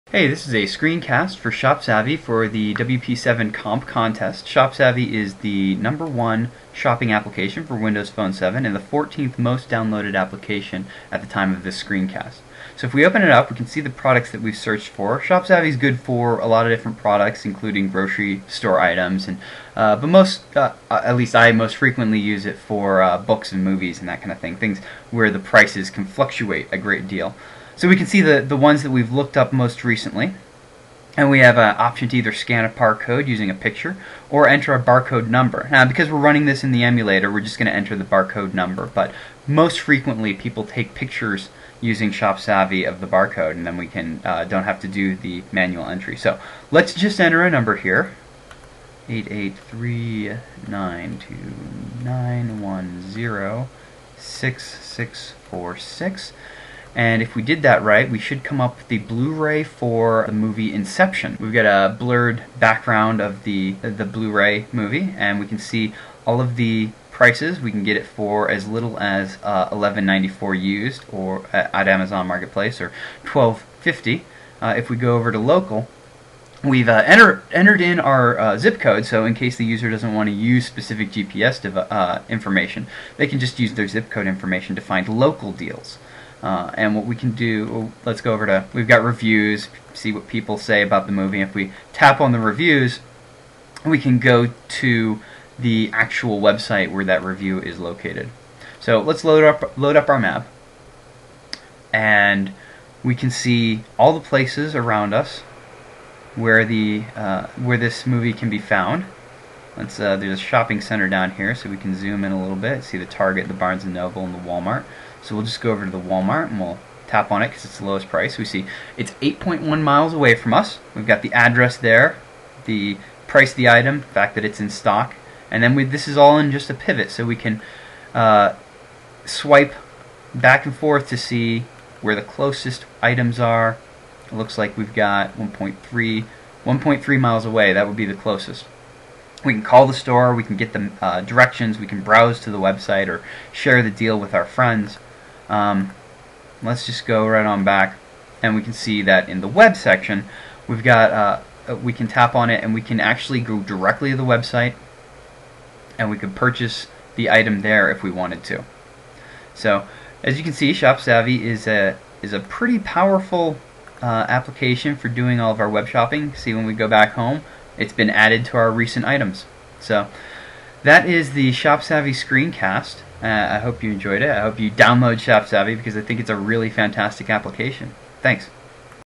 ShopSavvy Screencast for WP7 Comp Contest
This is a emulator screencast of ShopSavvy for the WP7 Comp Contest.